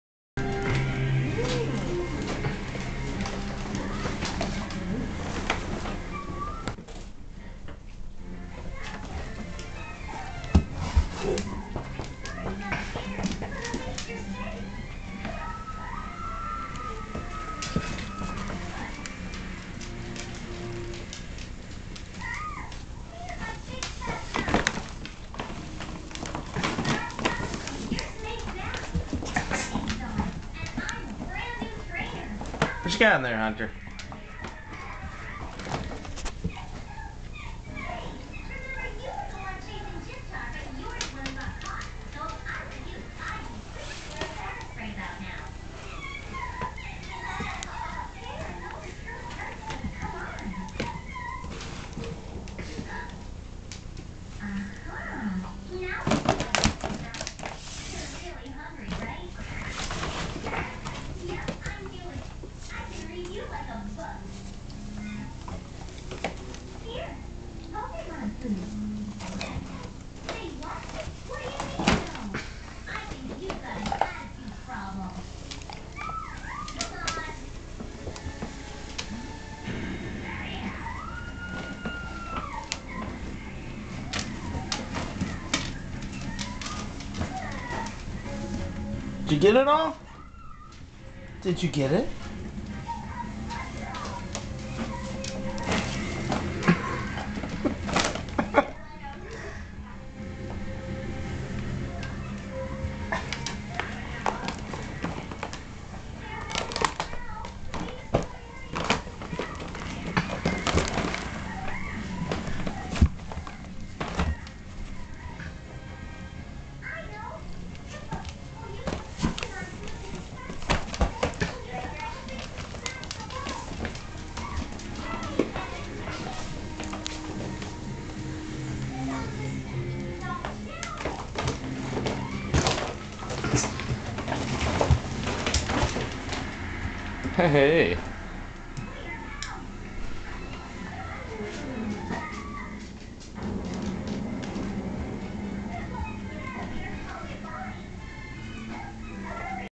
The Welsh Springer and the Food Bag.
Our Welsh Springer trying to get the last nugget from the bottom of the empty kibble bag.